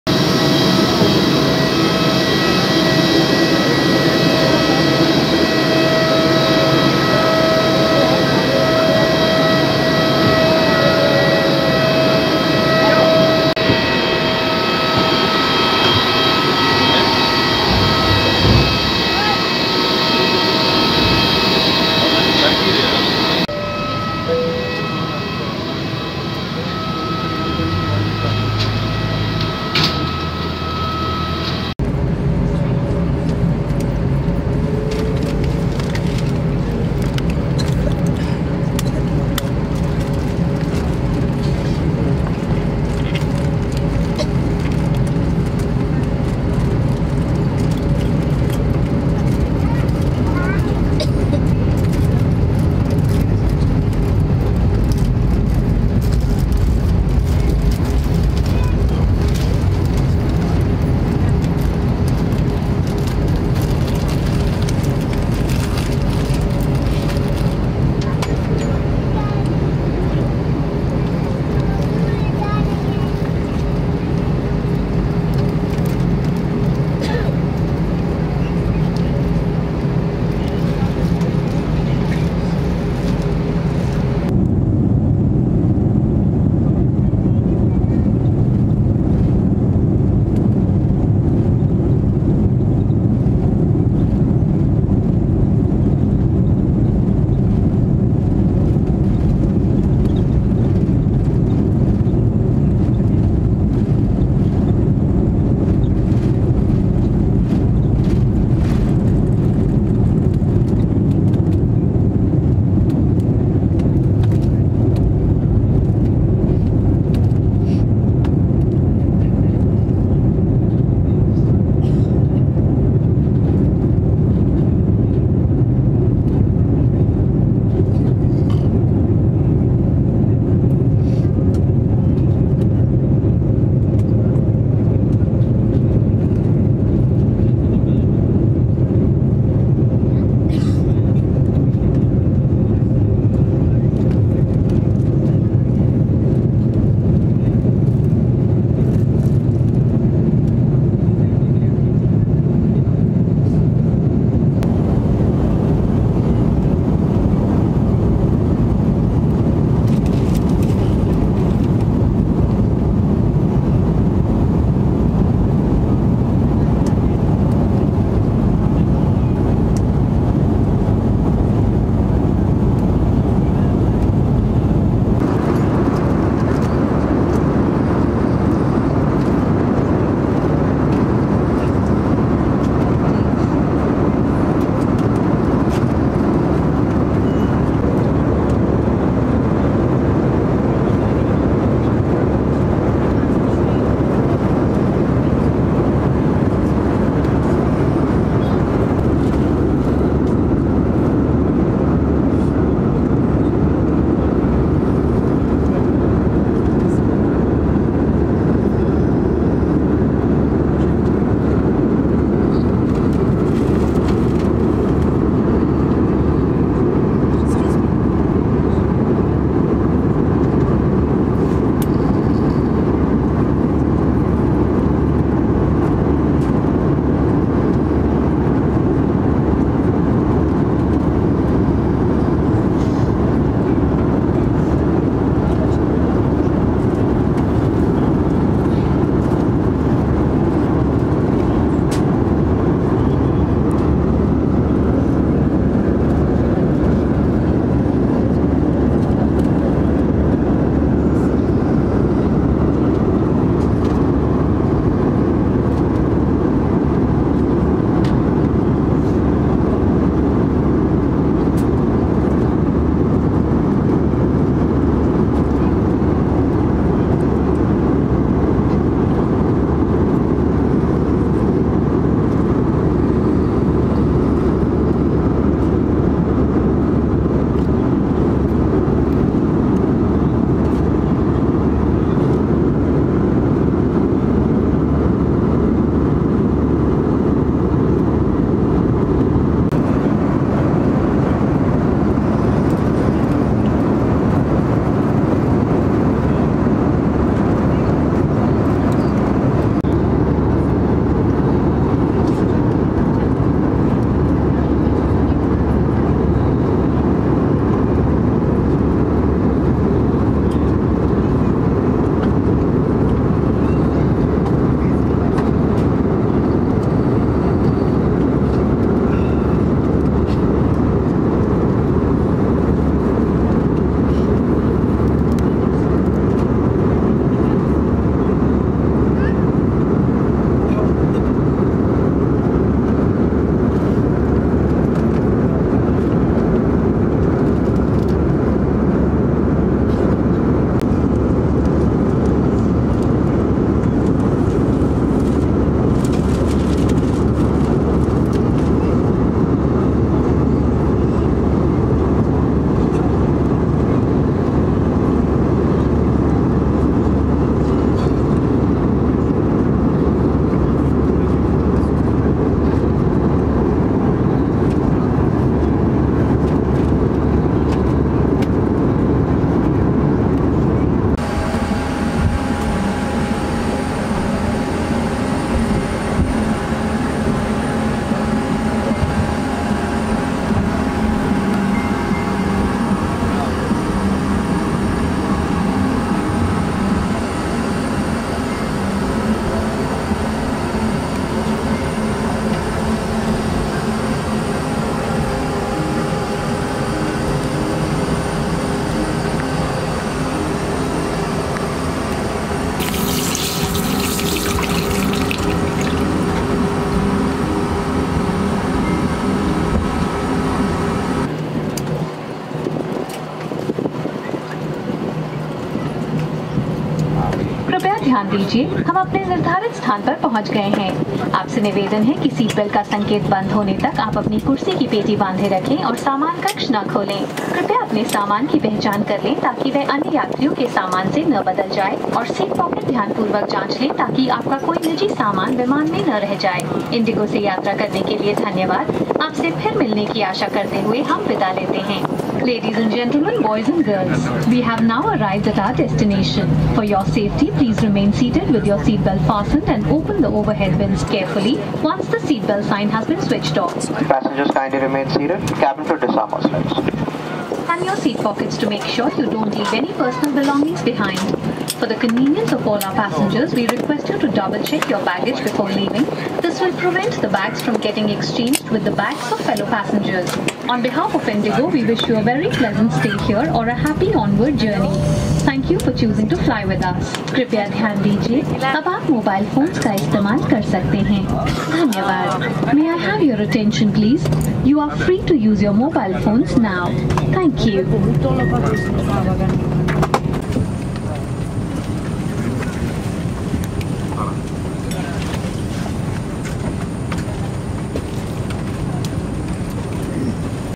My Journey with IndiGo Airlines from New Delhi | Complete Flight Report
I recently travelled by IndiGo airlines. Indigo Airlines is a low-cost carrier in India and the airline has the maximum market share in the country's aviation market. My destination was Dibrugarh Airport in Assam. The flight took off on time and reached the destination on time. The flight had a layover at Kolkata airport for 35 minutes. There is no complimentary food on Indigo airlines since it is a budget carrier. I hope you'll enjoy the video.